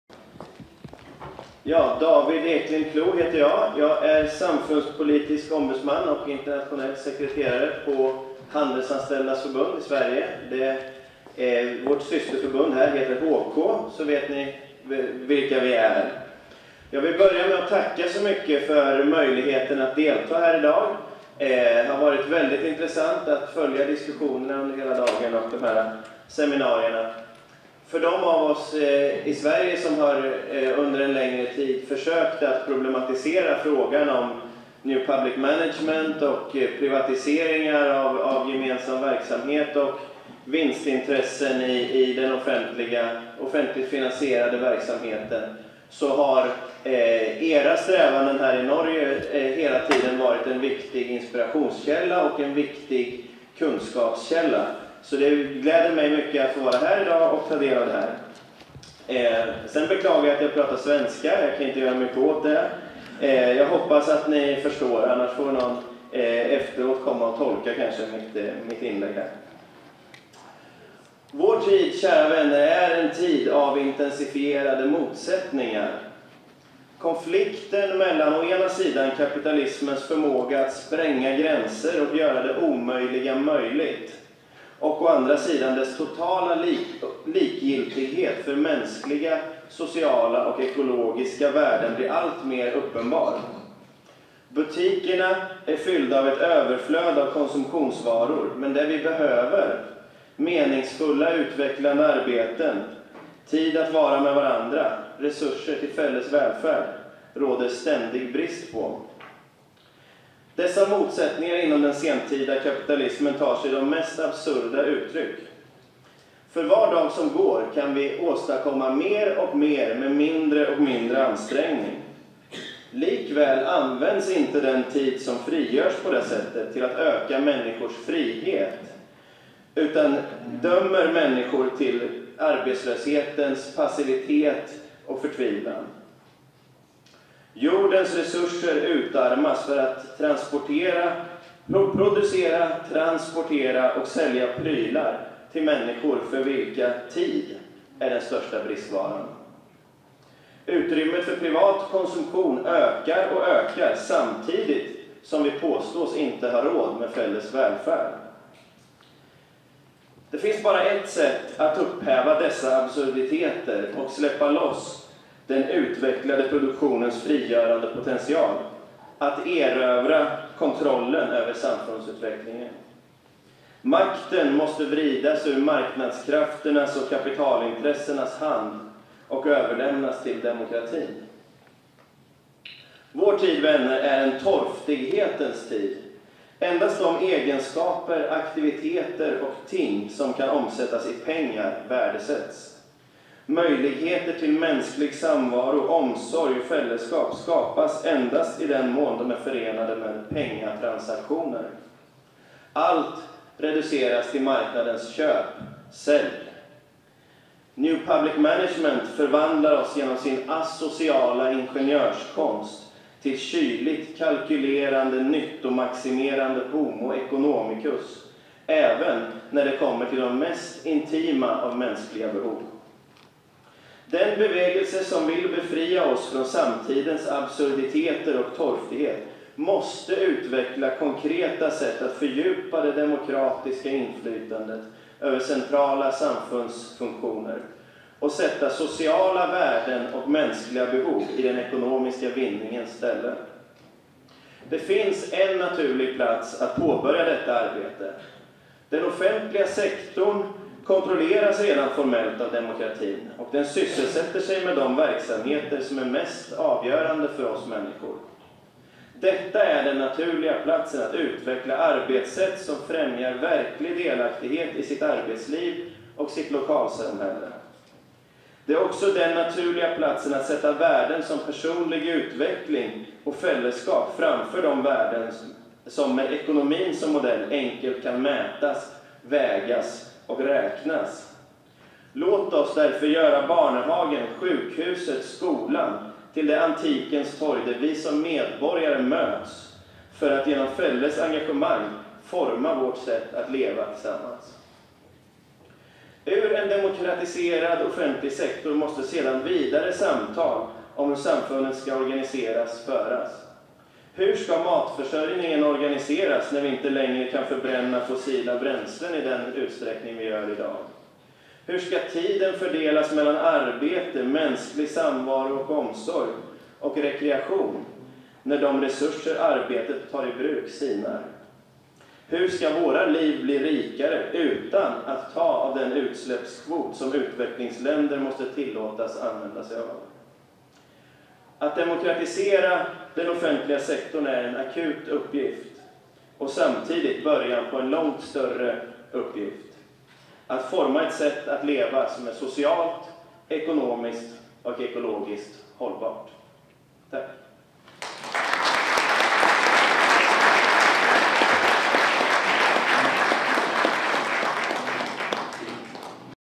8.seksjon Velferdsstaten David Eklind Kloo:"Fra defensiv til offensiv - vi har alternativene"
David er internasjonal sekretær og samfunnspolitisk ombudsmann i Handelsanställdas Förbund i Sverige. Han sitter også i redaksjonen til det sosialdemokratiske idémagasinet "Tidens". Han understreker at når velferdsstaten organiseres etter markedslogikk, framfor demokratiske prinsipp, går vi i retning av individualistiske, asosiale medborgere som konkurrere framfor å samarbeide.